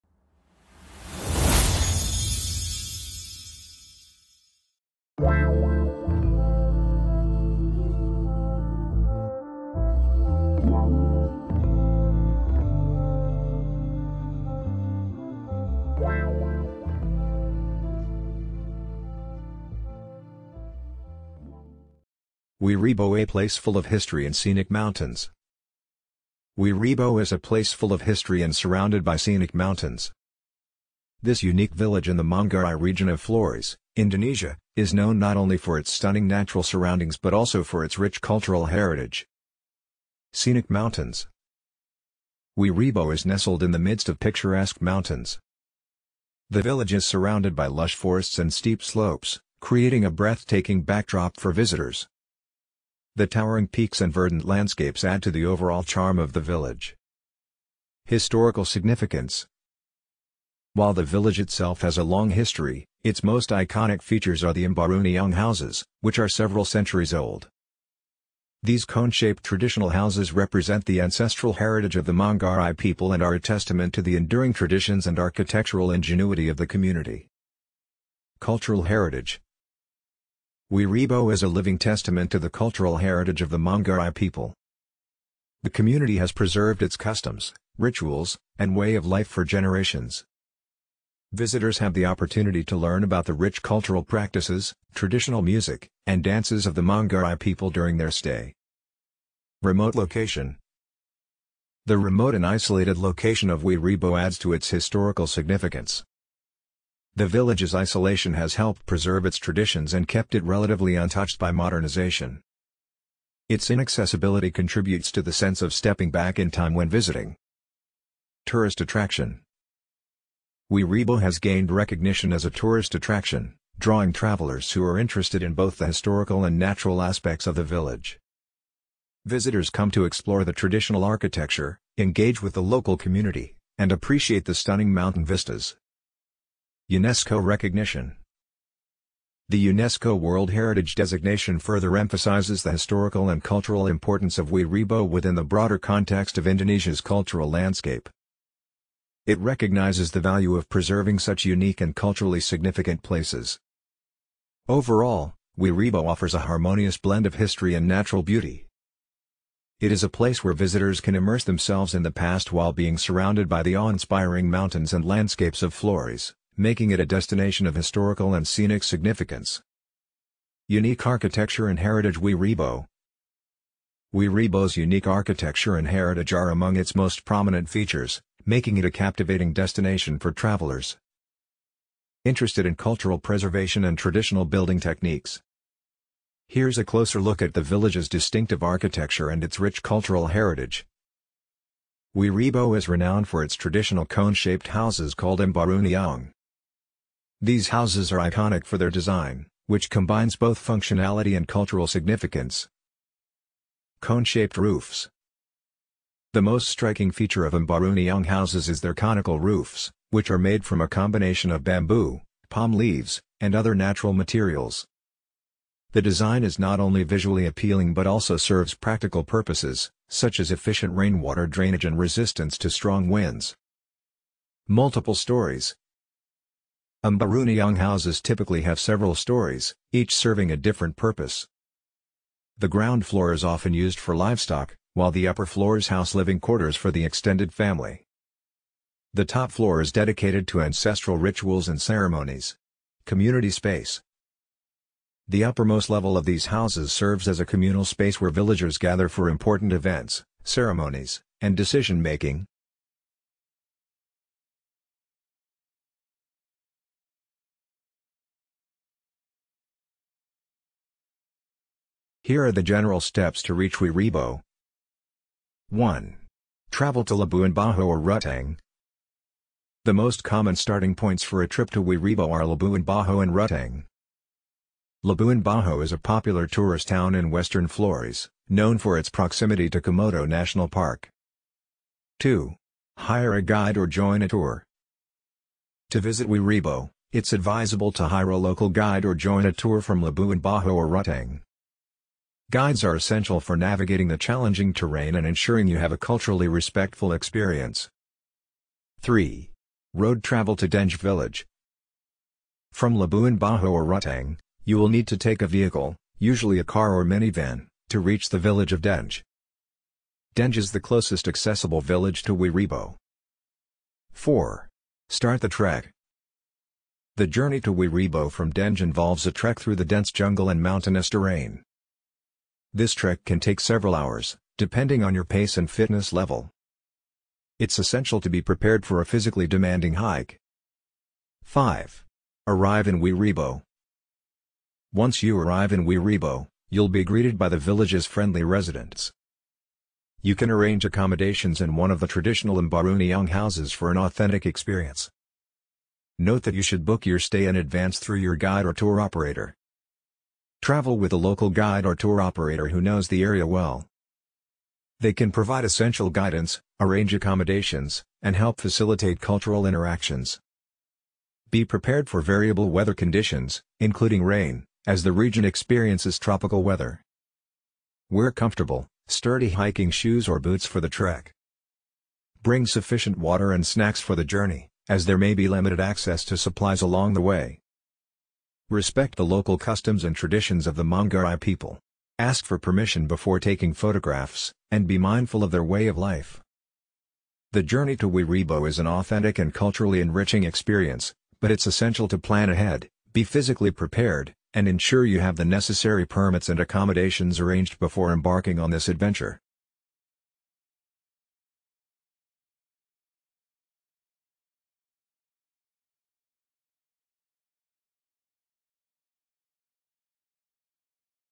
Wae Rebo - Indonesia [Amazing Places 4K]
Discover the enchanting village of Wae Rebo in Indonesia, nestled amidst lush forests. Experience traditional Mbaru Niang houses, warm hospitality, and a glimpse into the unique Manggarai culture. A hidden gem awaits.
Media Platforms
Content creator